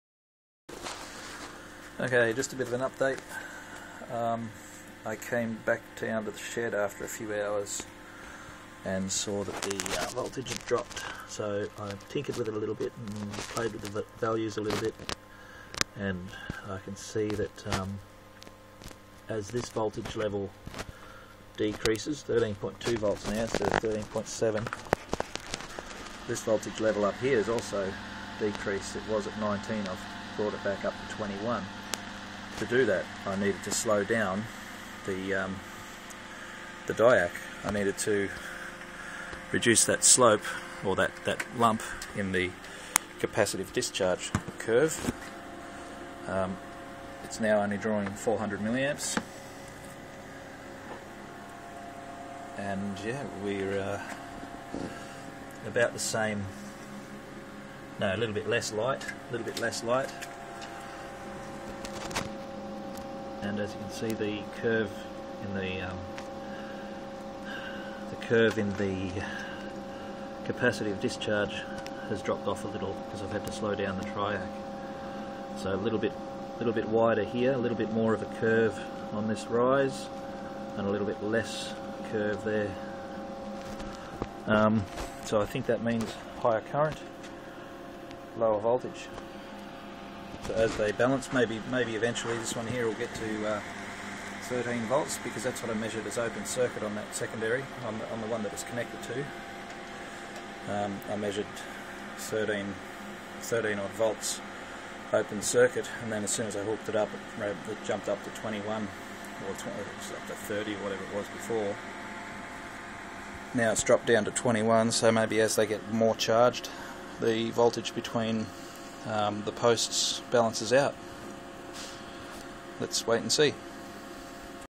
Capacitor battery charging
Follow up a few hours later. Trying to understand management of power levels to achieve best perfomance.